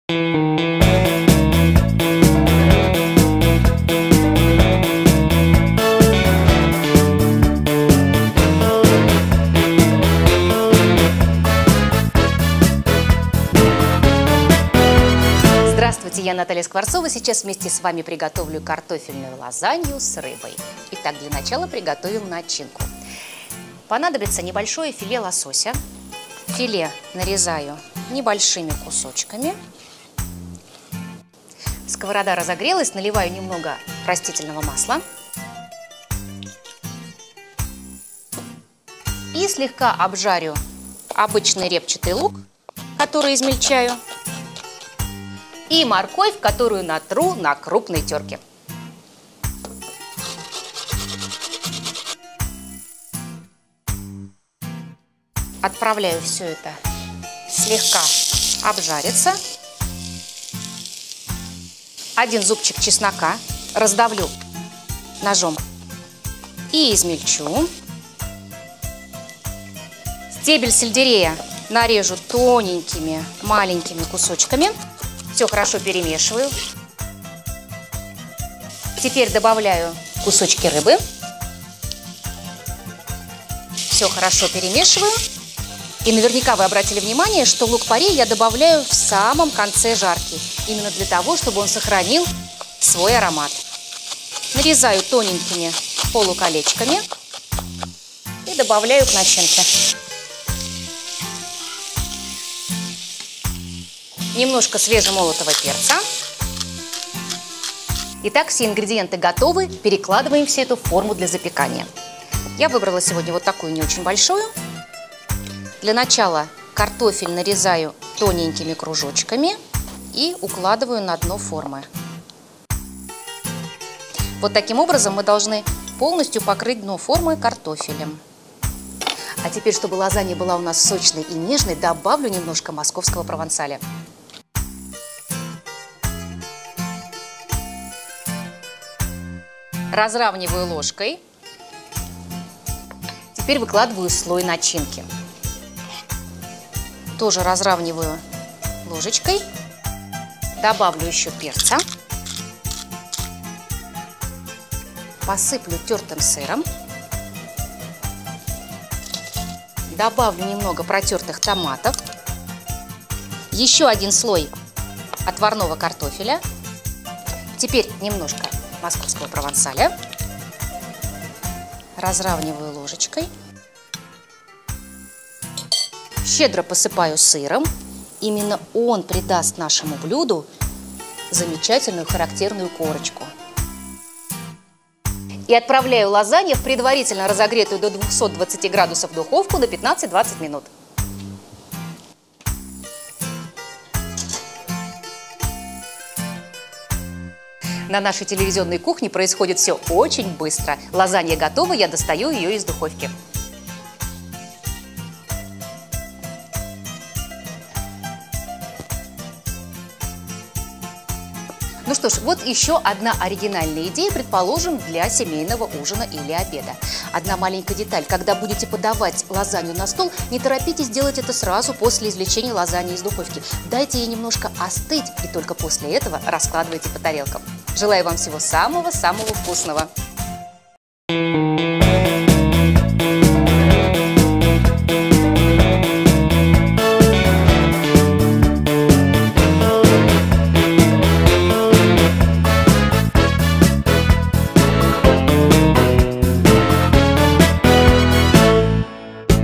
Любимые рецепты. Картофельная лазанья с рыбой.
Восхитительный рецепт картофельной лазаньи с рыбой.
 Хотя блюдо, как полагают, возникло в Италии, само слово «lasagna» происходит от греческого  («ласана»), что означает «горячие пластины». Современная лазанья готовится, как правило, из шести слоёв теста, при этом на каждый слой кладут мясной фарш, грибы, овощи, а сверху — тёртый сыр и несколько кусочков масла.